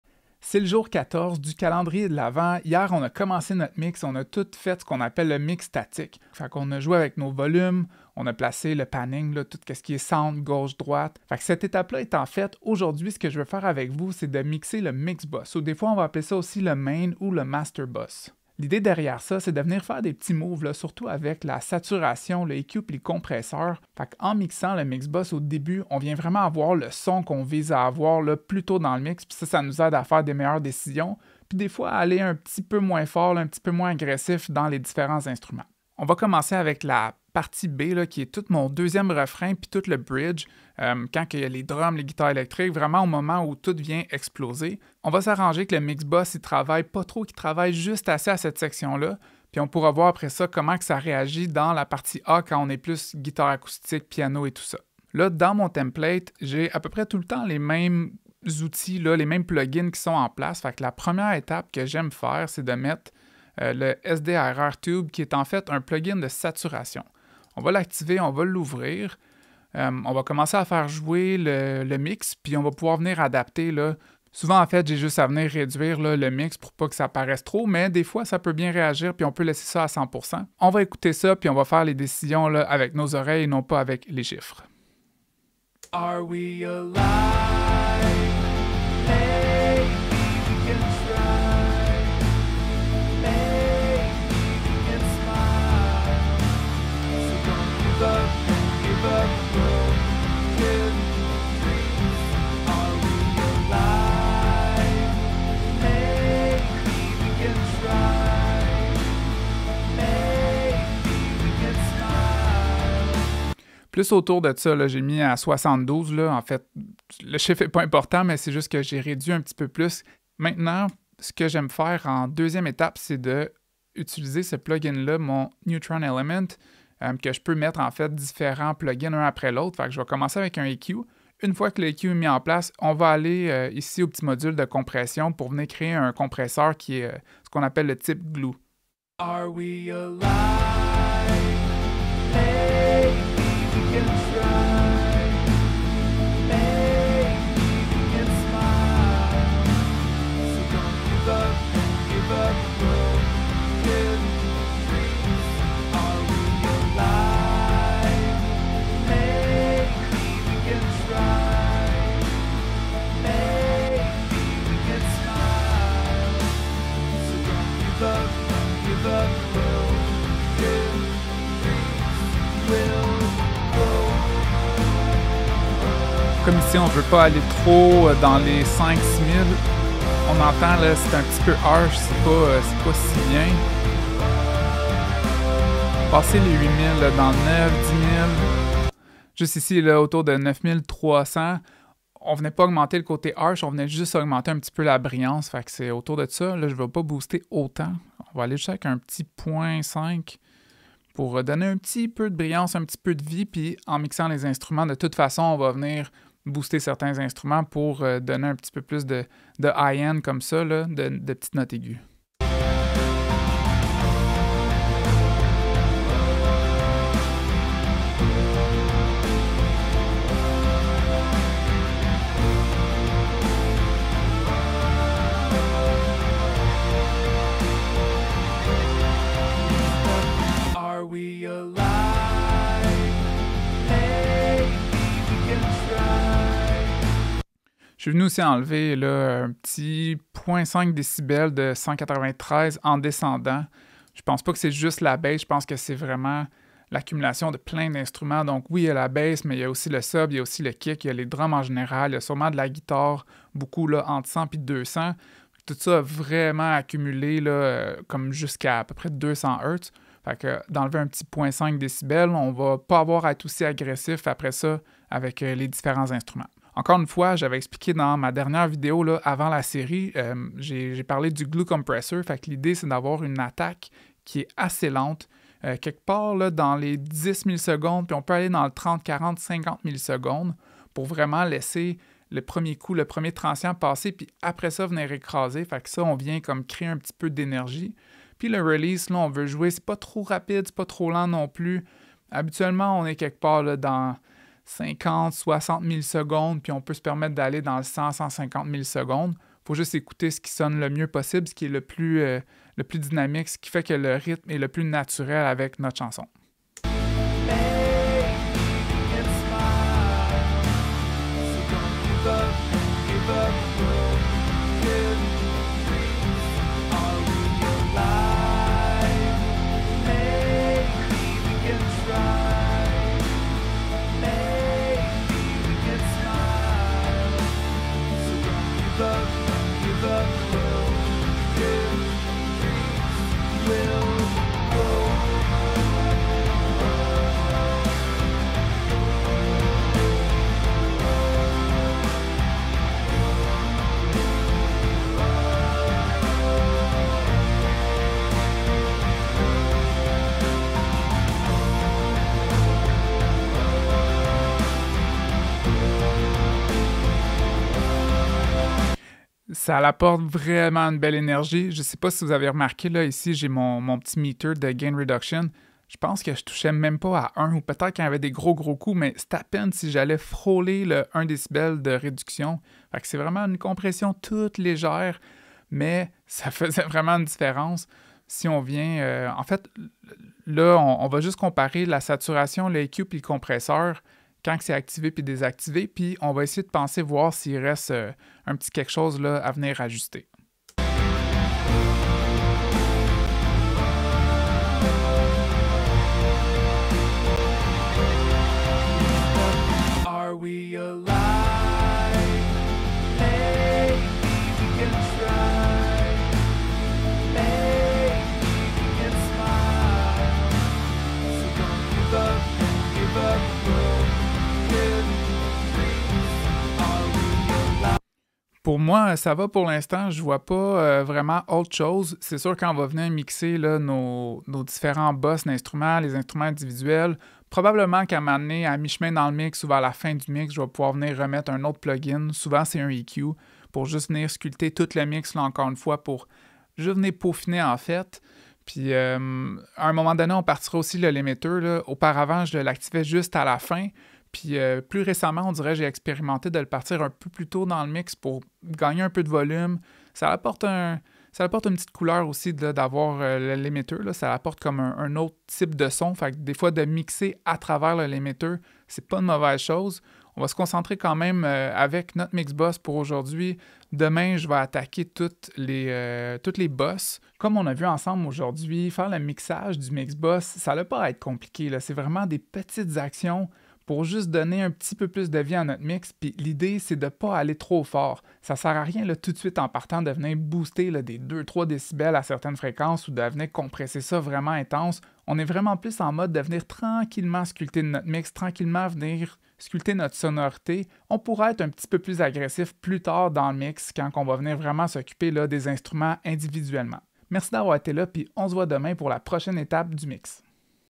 Mes 3 étapes pour le mixage du MIX BUS | JOUR 14
Obtenir gratuitement les pistes de la chanson (et d'autres surprises) pour vous pratiquer à mixer

Pour le jour 14 du calendrier de l'avent, je vous montre comment je fais le mixage du mix bus avant de commencer à toucher aux instruments. C'est la méthode du "top down mixing". L'idée est de faire quelques petites manipulations pour améliorer notre son globalement à travers toute la chanson, ce qui permet d'être plus doux sur les pistes individuelles par après!

Saviez-vous que j'ai mis en place mon propre cours en ligne? Pour en savoir plus